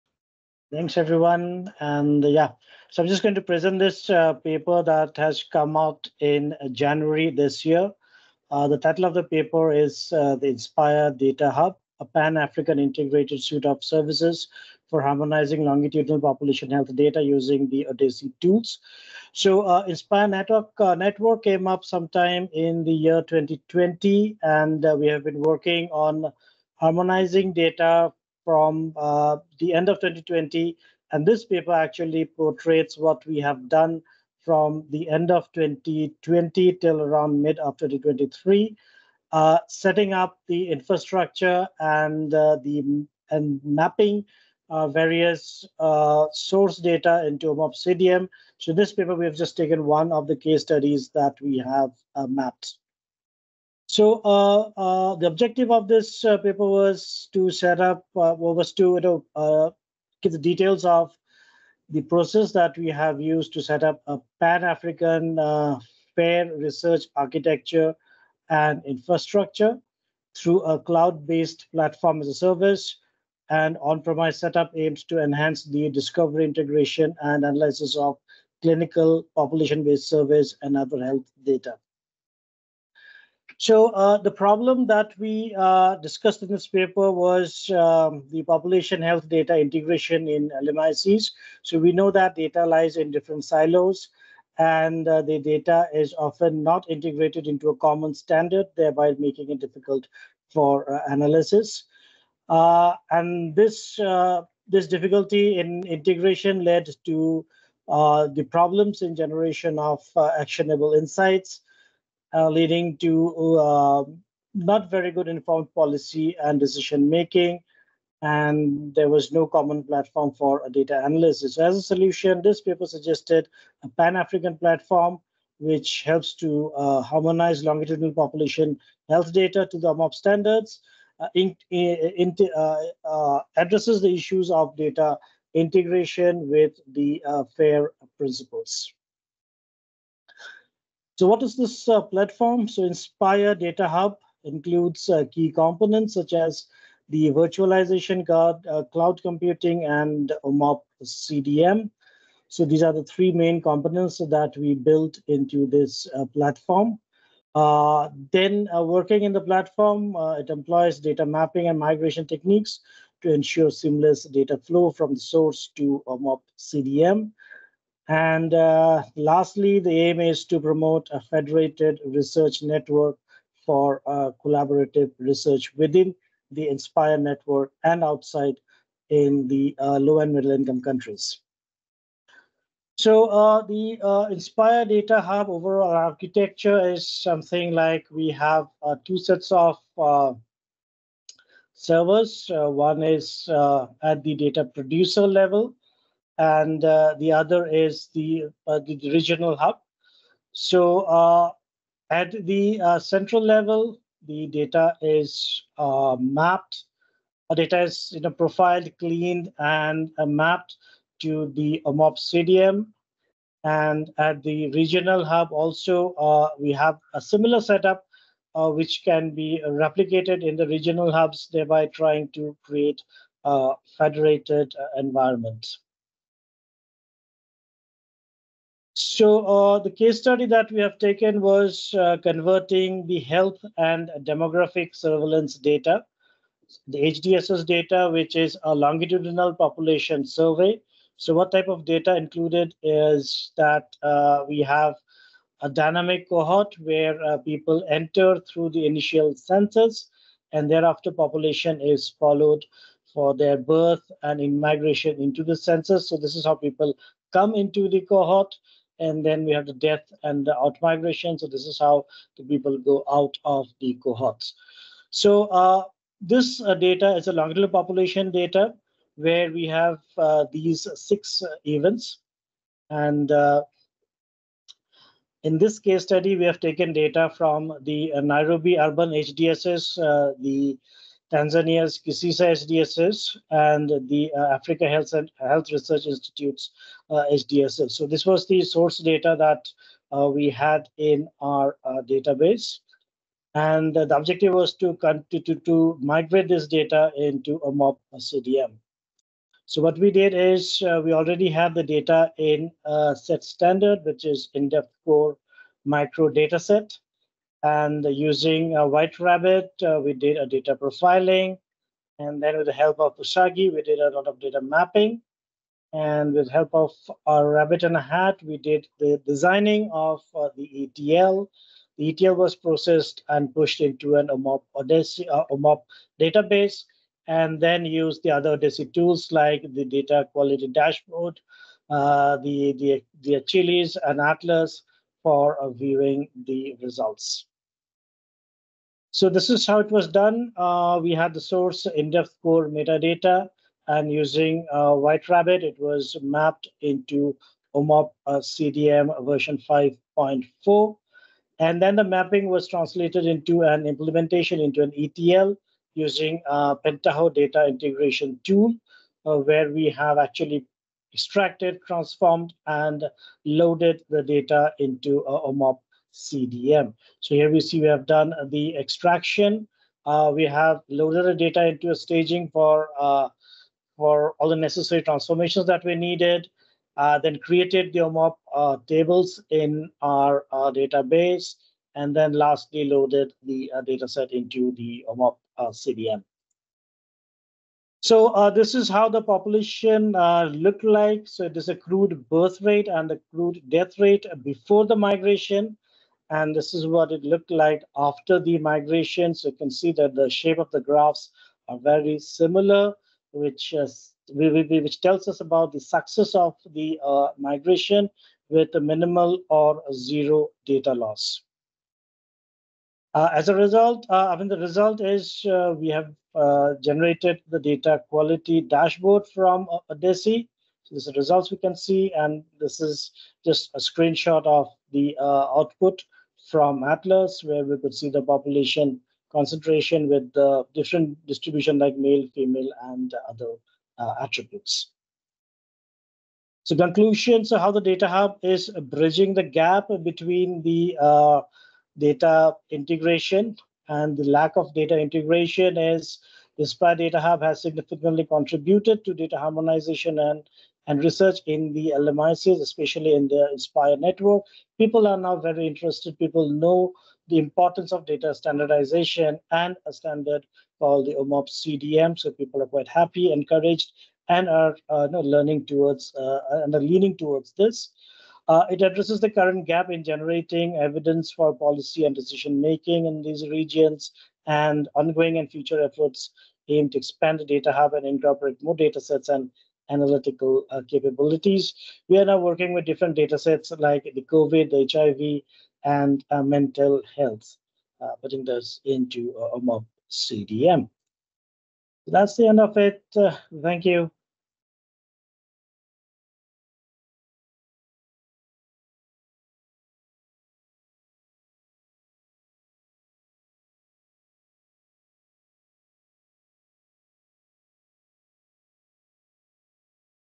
INSPIRE datahub: a pan-African integrated suite of services for harmonising ... (Bhattacharjee)
The March 26 community call focused on recent publications from the OHDSI community.

Title: INSPIRE datahub: a pan-African integrated suite of services for harmonising longitudinal population health data using OHDSI tools

Presenter: Tathagata Bhattacharjee • University of London

Recordings of all past Community Call presentations, as well as links shared during all updates, are posted